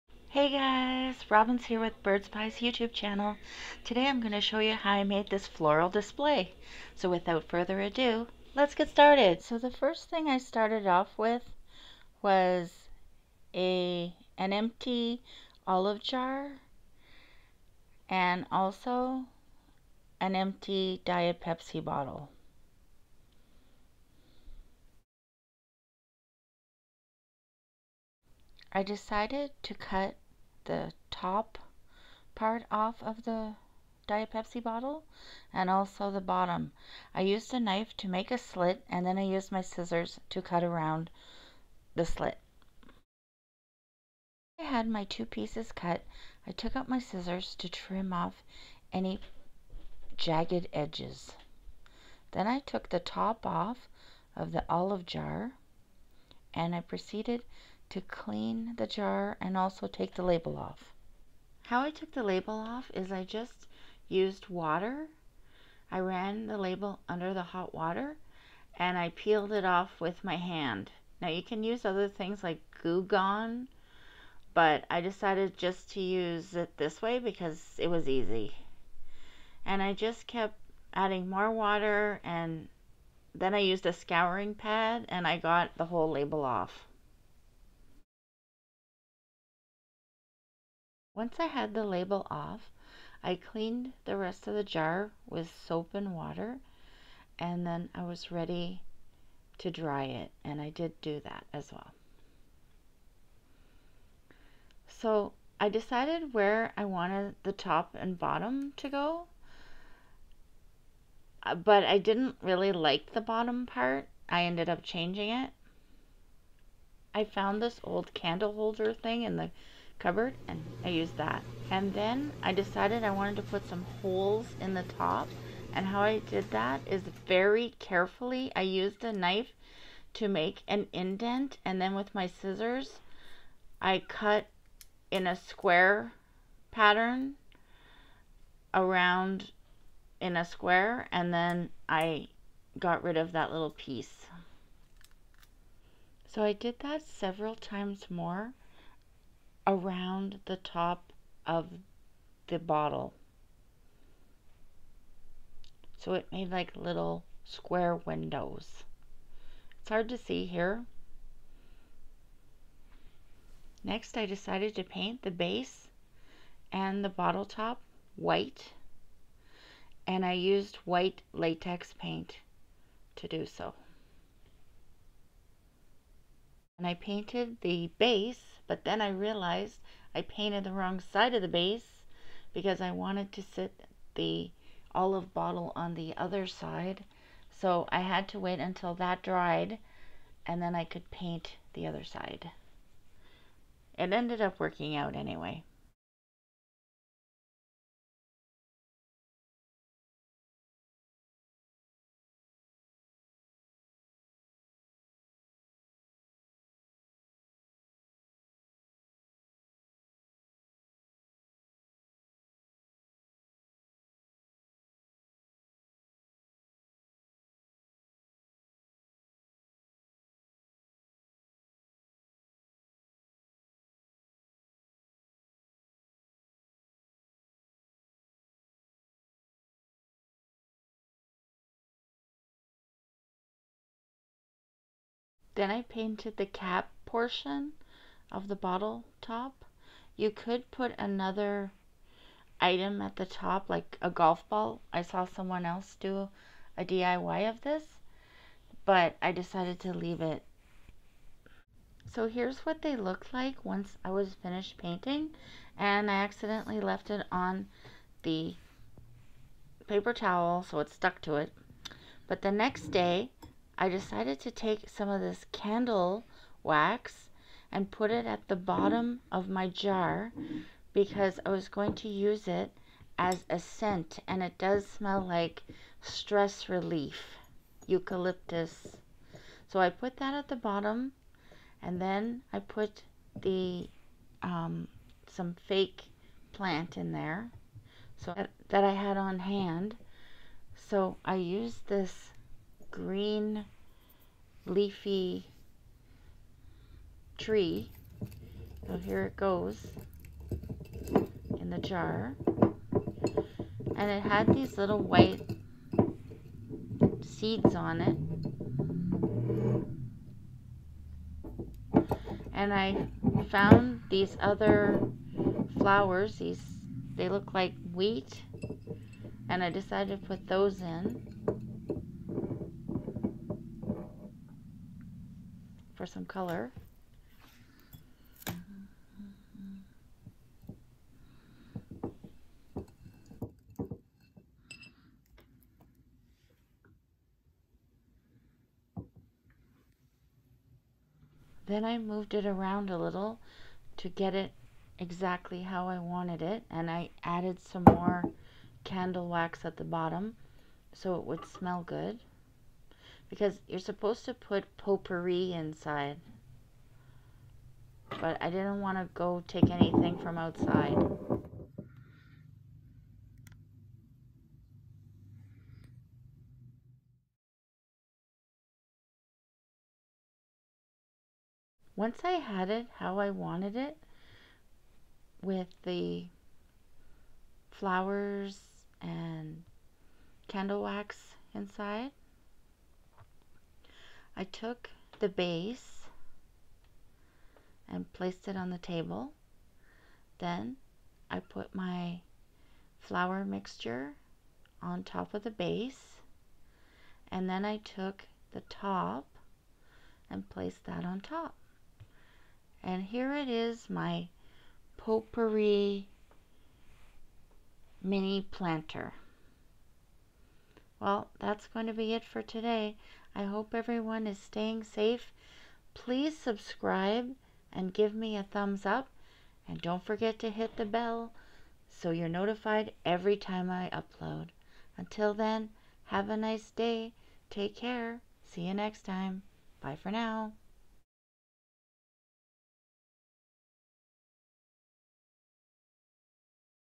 DIY Floral Scent Decor
Watch as I make an easy decor piece for my home with a stress relief scent.